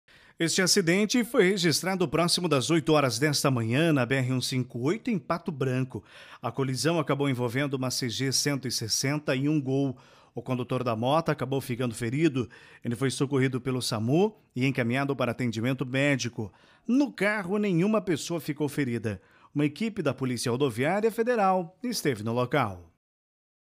Acidente deixa uma pessoa ferida nesta manhã (30) em Pato Branco
Segurança: O acidente ocorreu na manhã de hoje (30) na BR-158 em Pato Branco.